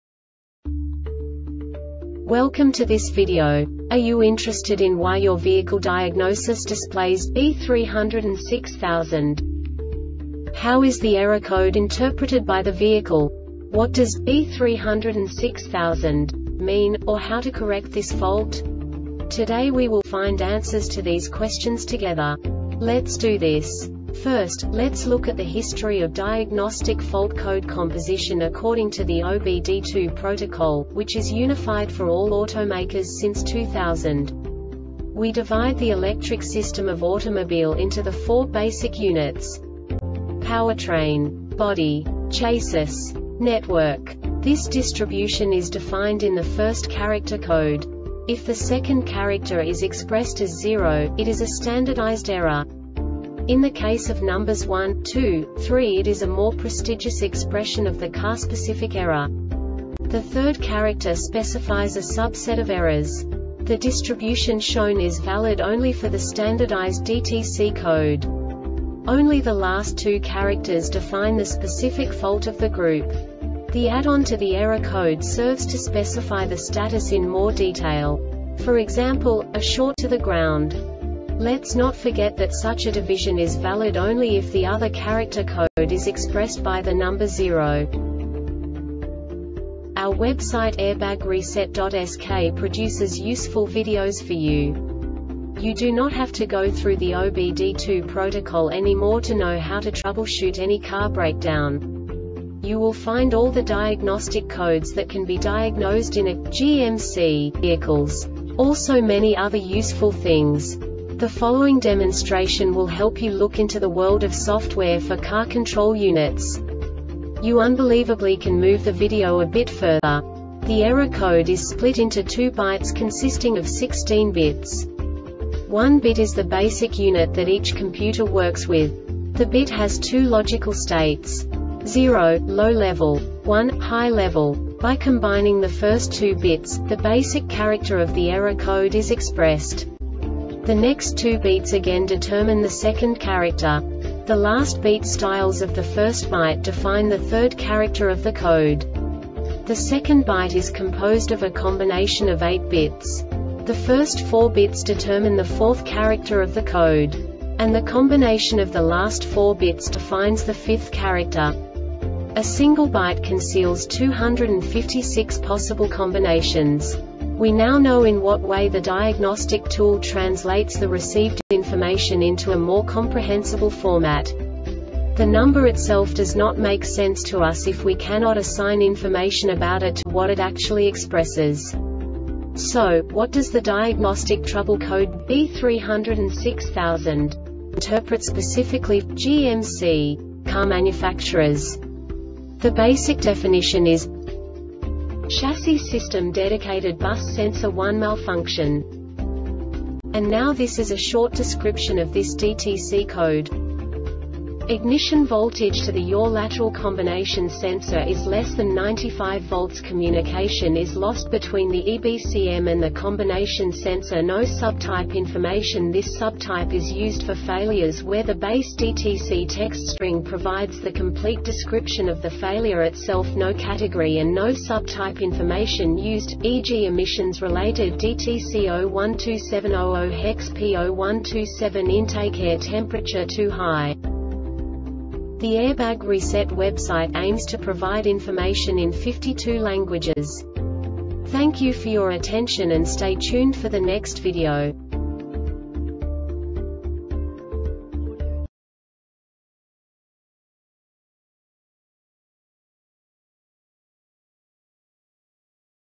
DTC GMC B3060-00 Short Explanation
Contents:
0:21 Basic DTC analysis according to OBD2 protocol standard.
1:48 Insight into programming
2:58 A diagnostically understandable description of the Diagnostic Trouble Code
3:05 Basic error definition

Make
GMC
Code
B3060 00
Definition
Chassis System Dedicated Bus Sensor 1 Malfunction
Cause
Ignition voltage to the yaw or lateral combination sensor is less than 95 voltsCommunication is lost between the EBCM and the combination sensor
Failure Type